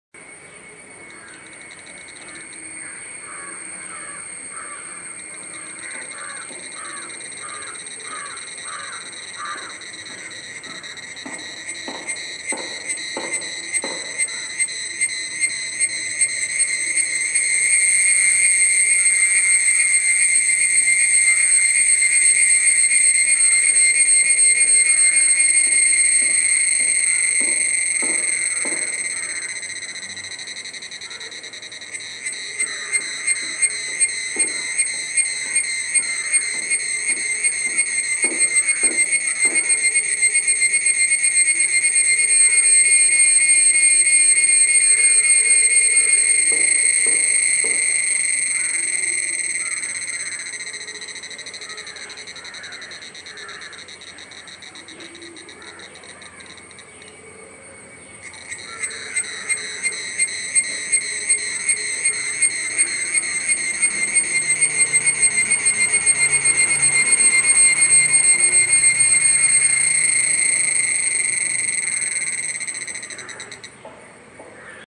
ഒരേ താളം ...
ചീവീടുകളുടെ പാട്ട് - ഒരേ താളത്തില്‍